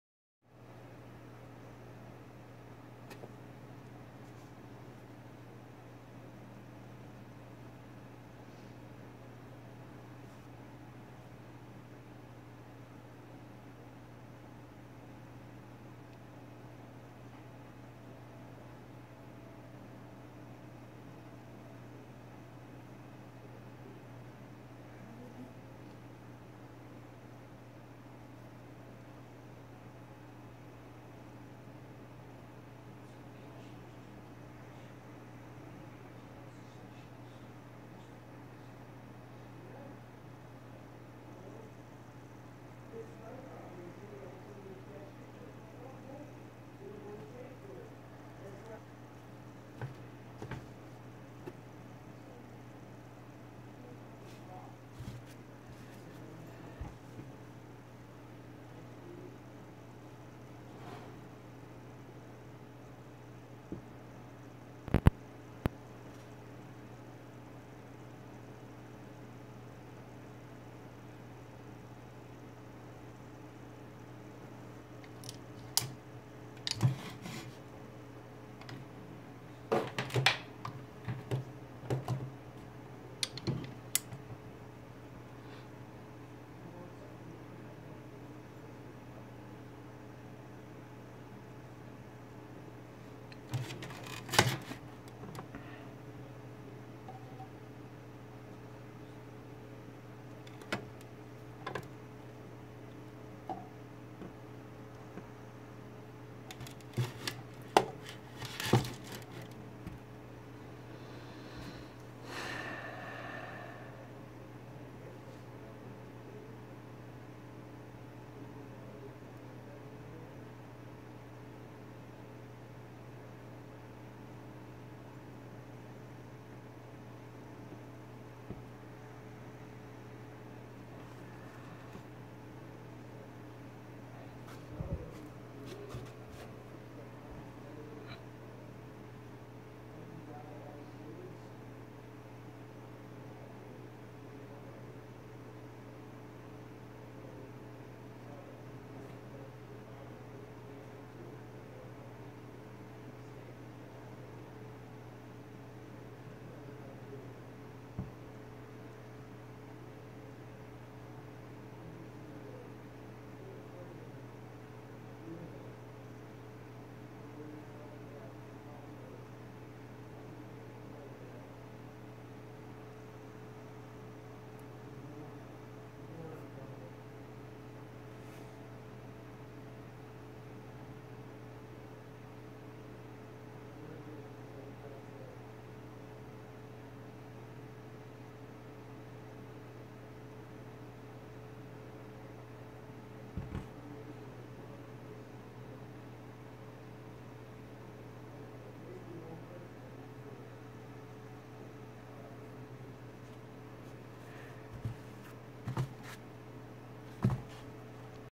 Lot of 3 Hemingray (2-piece Transposition, D-990)
A closer look at this lot of 3 Hemingrays. Included are a complete 2-piece Transposition and D990. CD 190/191 & 127.